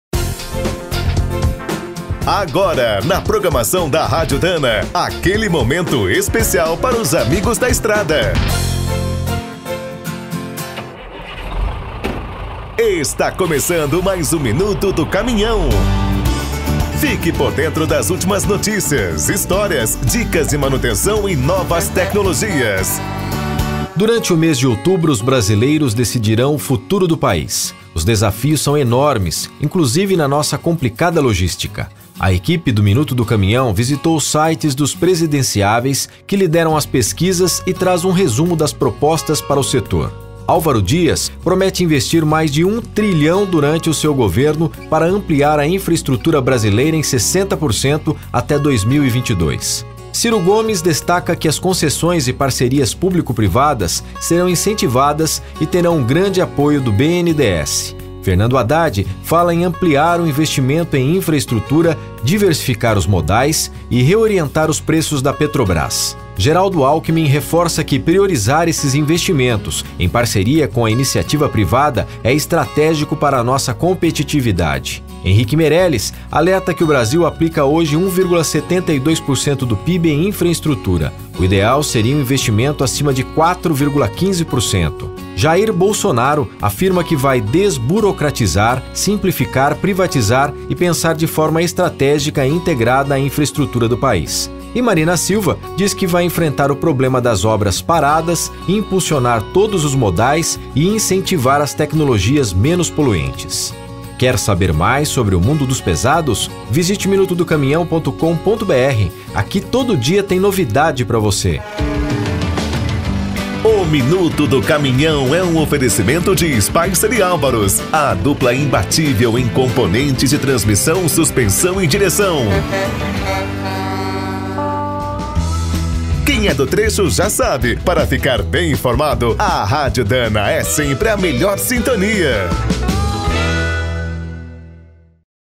455 - Minuto Hoje - As Eleições e Os Transportes
Conheça as propostas dos presidenciáveis que lideram as pesquisas para melhorar a nossa logística.